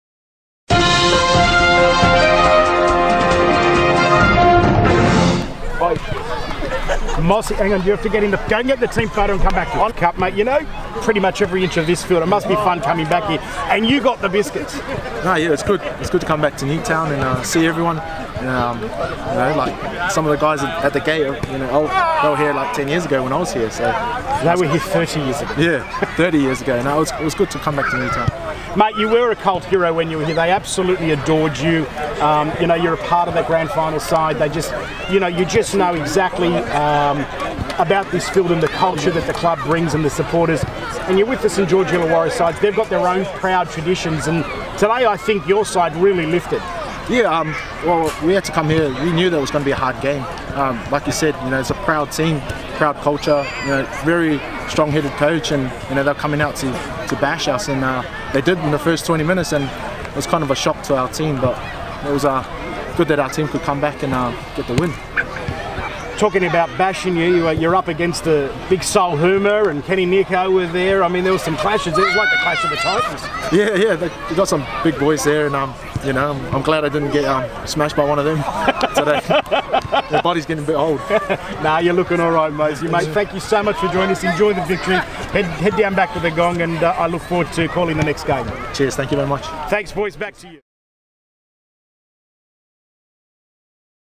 Steele Sports Post Match Interview Mose Masoe Rnd 11
Illawarra front rower, Mose Masoe, catches up with Steele Sports after Illawarra's win over Newtown in Round 11 of the Intrust Super Premiership.